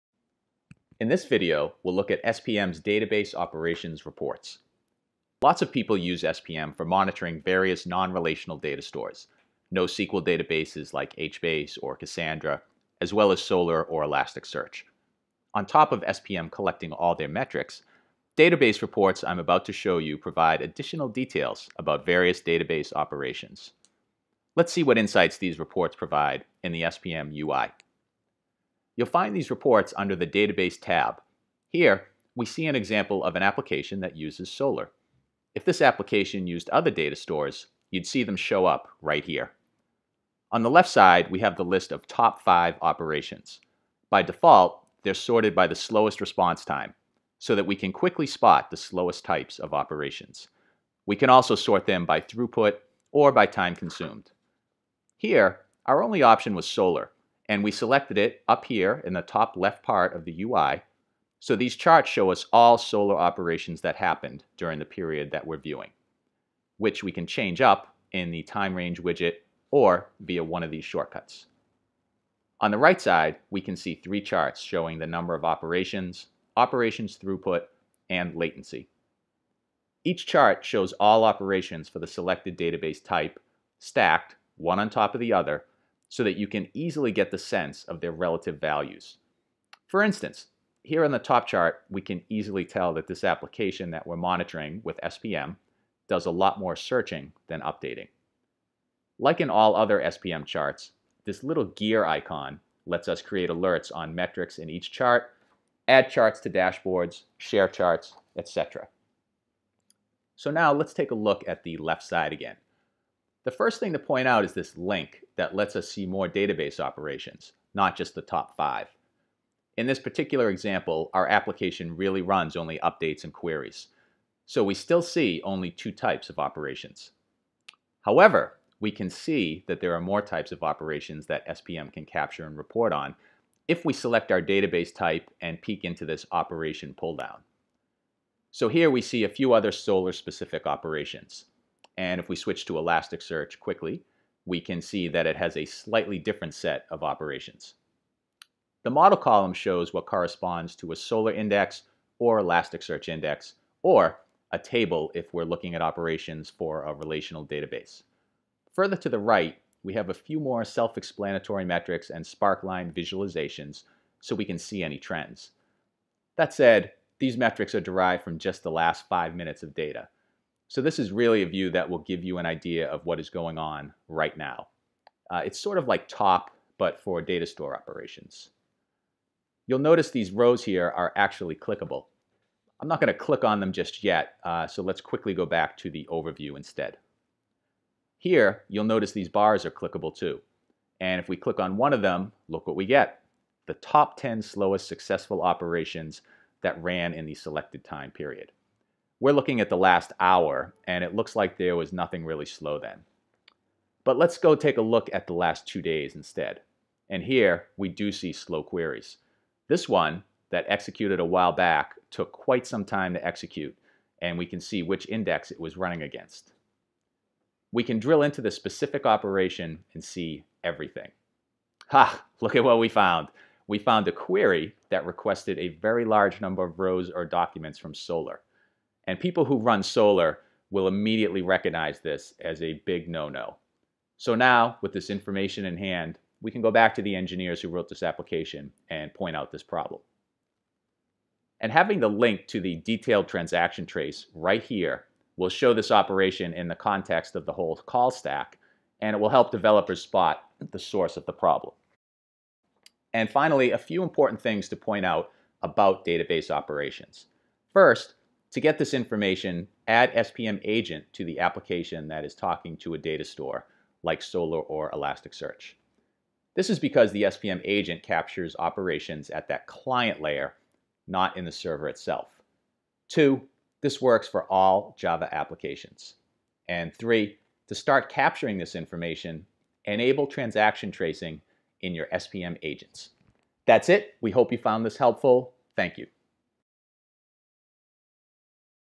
Introducing Top Database Operations
Top Database Operations in SPM gives you valuable intelligence on IT Ops data.  This includes: top 5 operation types across all your data stores or filtered to a specific data store type; top 5 operation types by speed, throughput, or simply their volume, time-series reports for volume, throughput, and latency broken down by operation type; and more.  to learn more.